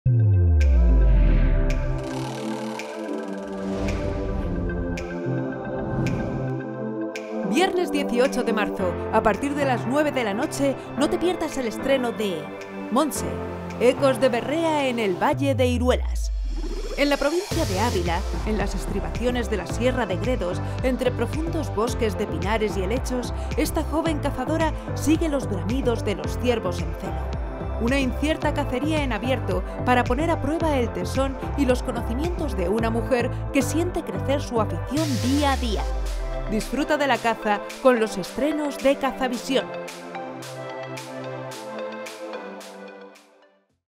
Montse DH. Ecos de berrea en el Valle de Iruelas.
En la provincia de Ávila, en las estribaciones de la Sierra de Gredos, entre profundos bosques de pinares y helechos, esta joven cazadora sigue los bramidos de los ciervos en celo. Una incierta cacería en abierto para poner a prueba el tesón y los conocimientos de una mujer que siente crecer su afición día a día.

+ vídeos
+ series
+ usabilidad
+ calidad
+ diversión